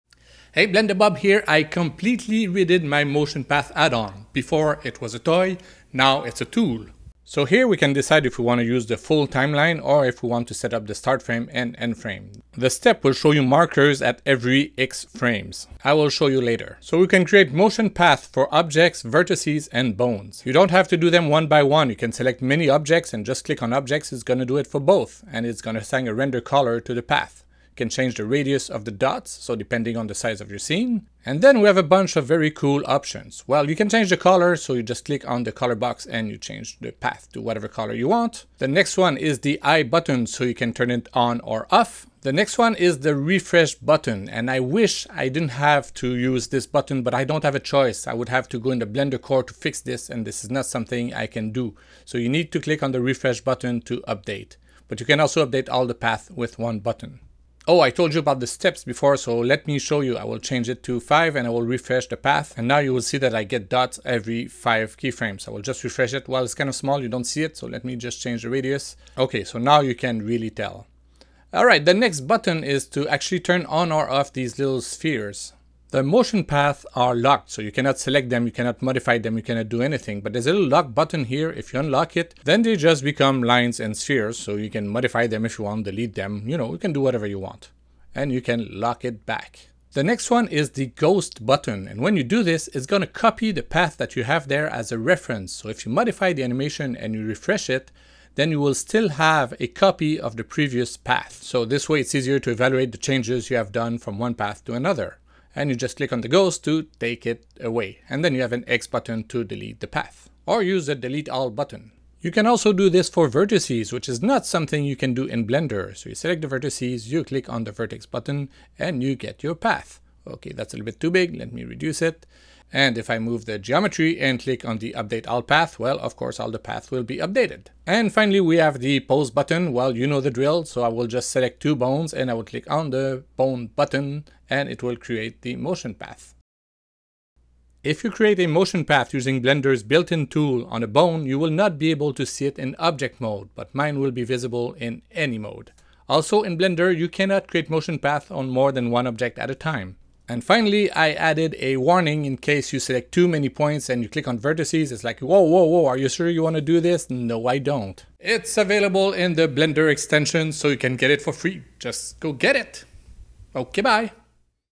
Motion Path manager for Blender
I added a button for displaying keyframes only!

Get it on the extension platform. It's free!